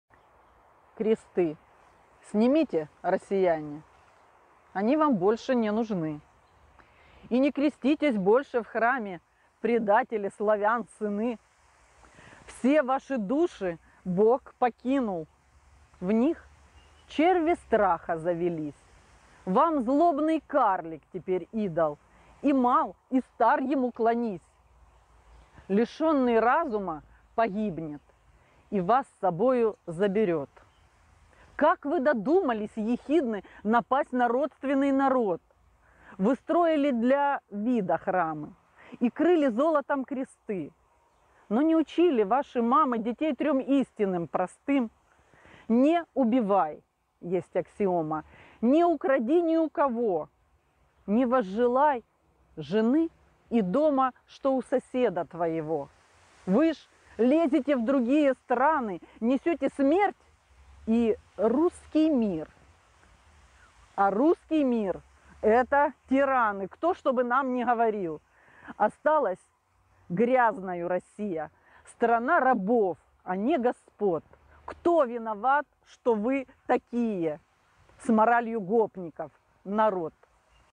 Кресты снимите, россияне
Вірш прочитала в газеті. Андрій Демент'єв був вказаний, як автор.
З юності люблю вірші Андрія Дмитровича і поважаю його як особистість. За моїм відчуттям вірш складений за стилем улюбленого поета. Однак на сайті «Український портал поезії» в публікації автор ХорАл запевняє читачів (дата публікації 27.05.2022 року), що це його вірш.

На іншому сайті автором вірша названий А.Ширков,  дата написання - 01.03.22.

А на одному з you tube каналів автором віршу вказаний  Юрій Андрієвський (відео від 16 квітня).

Оскільки не знаю напевно, кто автор вірша, який висловлює стан душі не тільки мій, а й усього народу України, я останні хвилини відео вирізала, бо автором вірша назвала Андрія Демент'єва.

Прошу автора, якщо він побачить це відео, написати мені, я обов'язково його вкажу.